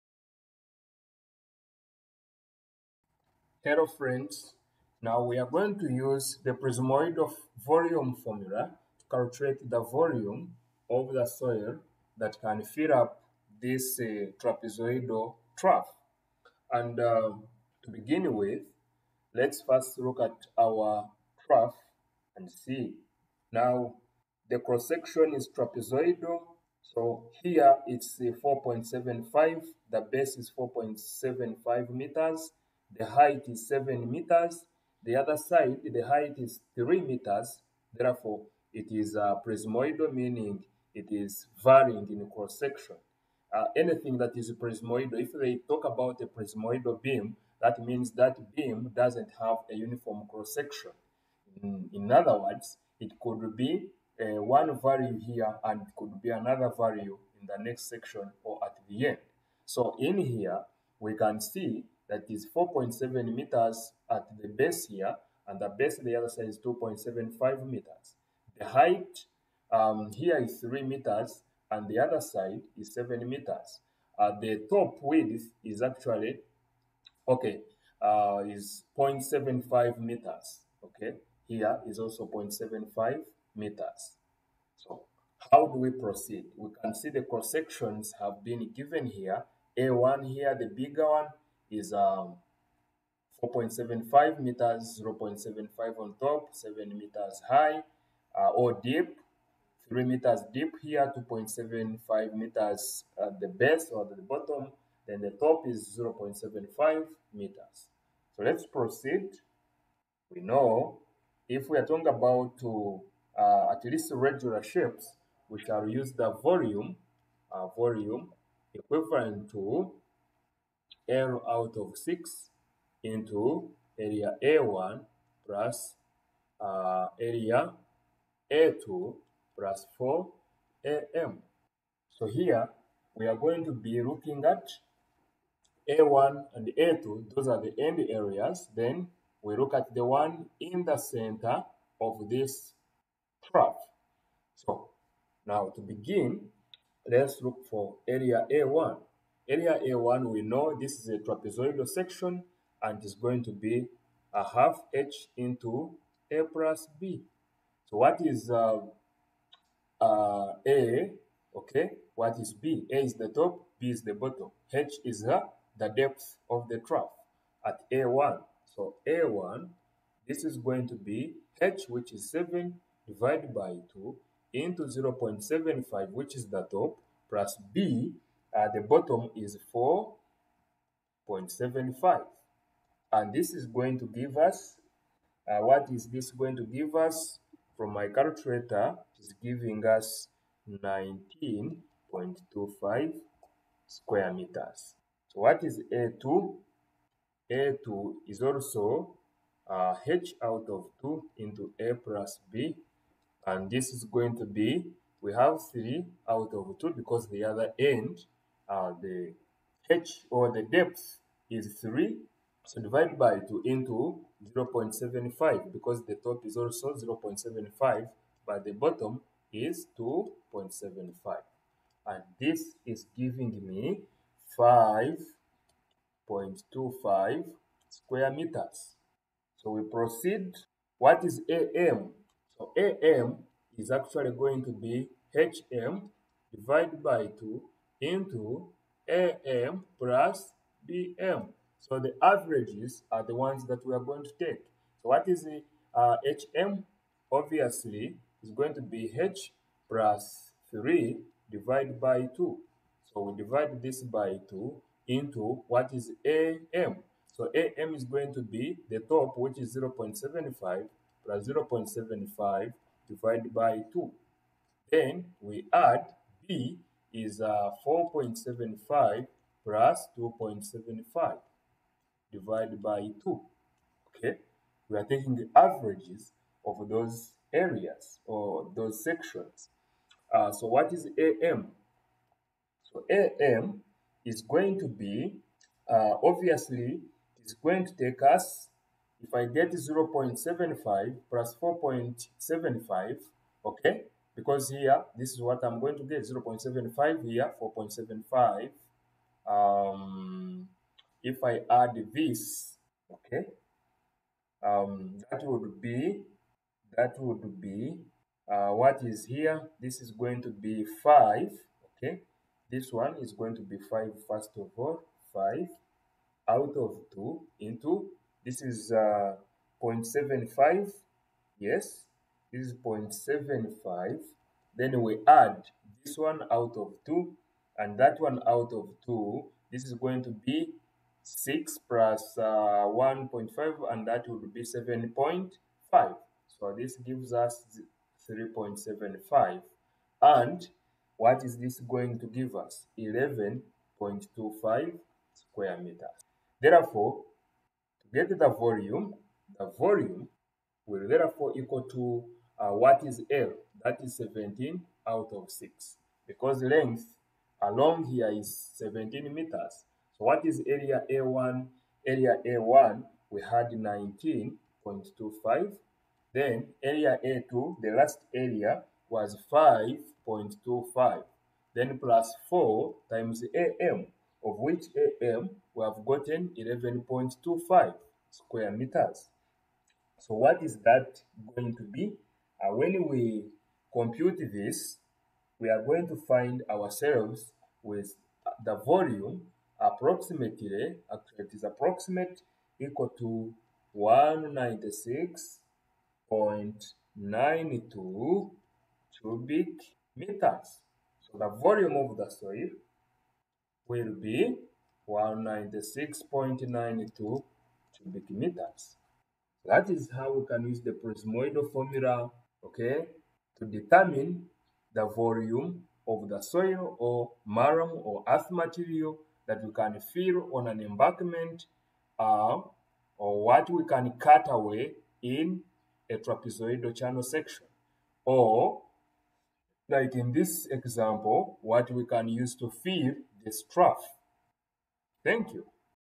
Prismoidal formular for volume || quantity of earthwork
Use the prismoidal formula to compute the volume of earth material excavated to achieve the channel section below